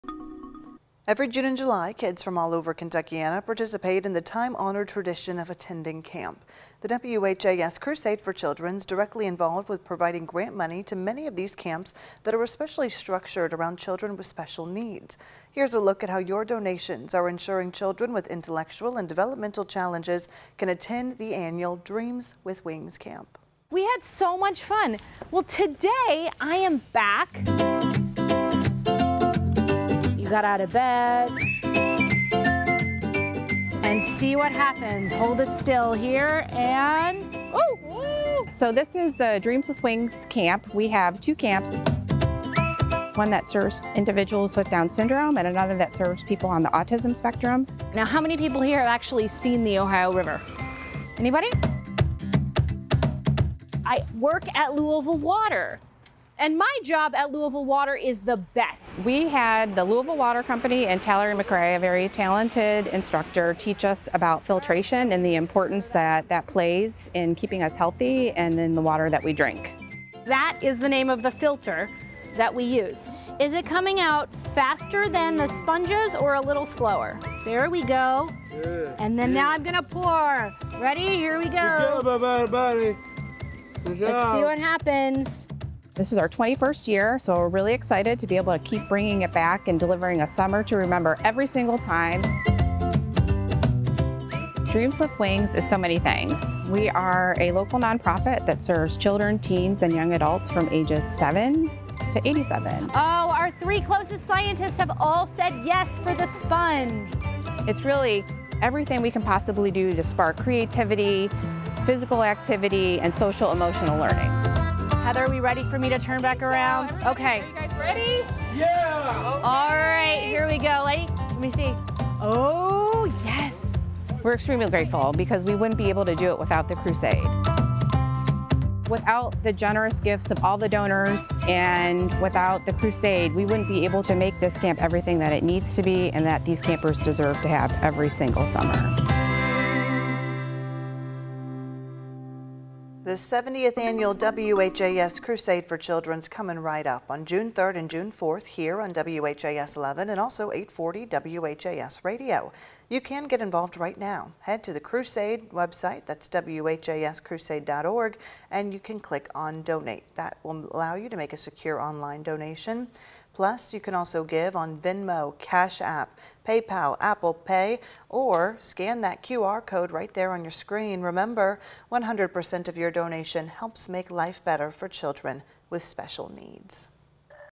How your donations help children with special needs attend camp
The 70th Annual WHAS Crusade for Children is coming up June 3 and June 4 on WHAS11 and 840 WHAS Radio.